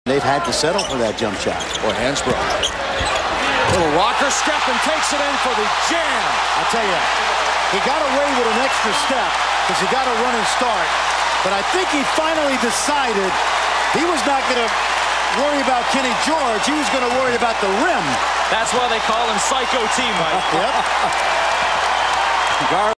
Tyler Hansbrough dunks on 7-9 Kenny George
Tyler dunks over UNCA 7'9" Kenny George at the Dean Dome 1-9-08